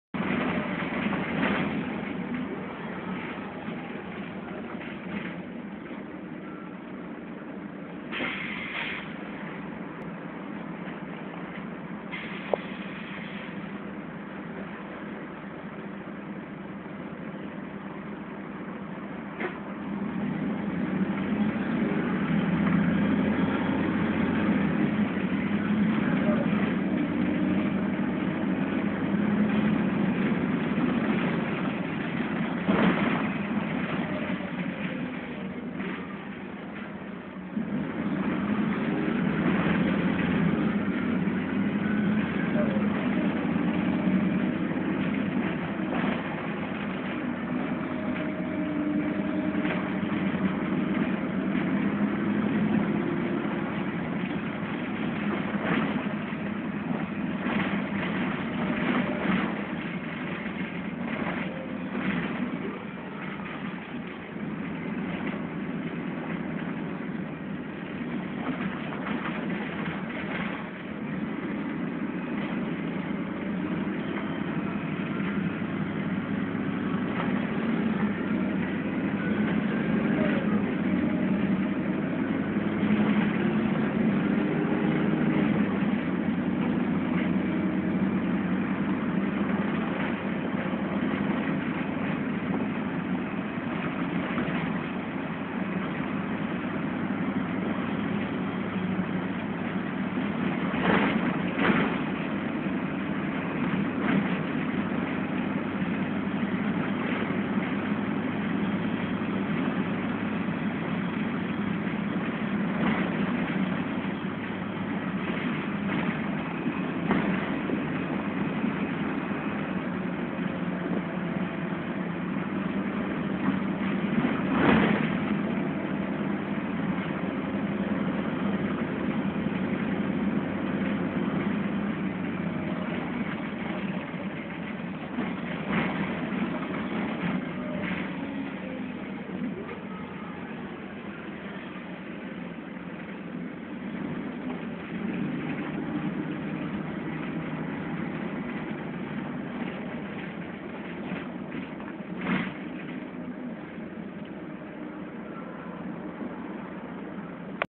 National Express west Midlands Volvo B6LE Wright Crusader 516 P516 EJW route 70 20.07.2012
516 is still in service despite being listed as withdrawn. Short clip